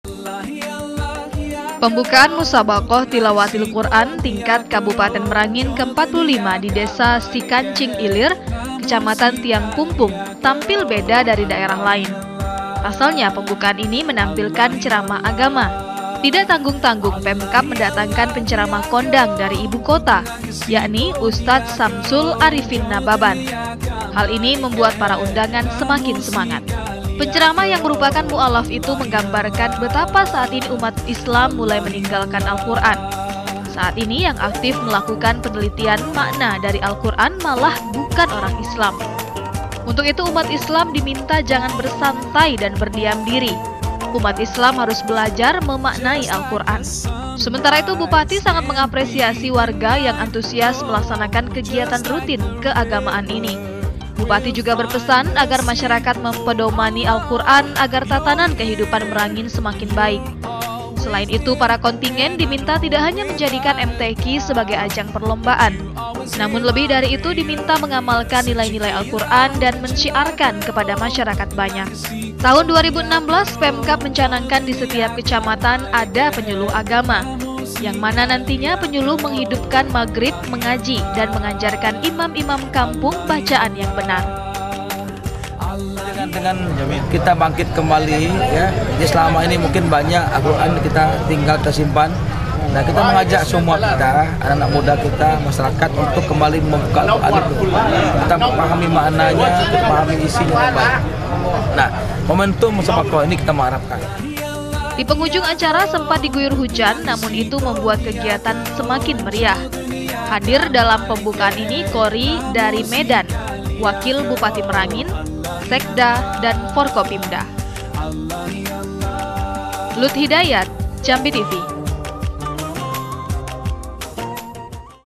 Pembukaan MTQ Ke- 45 Di Merangin Berlangsung Khidmat Dan Spektakuler
Musabaqah Tilawatil Qur’an MTQ tingkat Kabupaten Merangin yang ke 45 secara resmi dimulai di Kecamatan Tiang Pumpung. Pembukaan dipimpin langsung Bupati Alharis yang berharap syiar agama Islam dapat terus dilakukan.

Facebook.com: Jambi-TV
Youtube.com: JambiTV Kebangaan Kito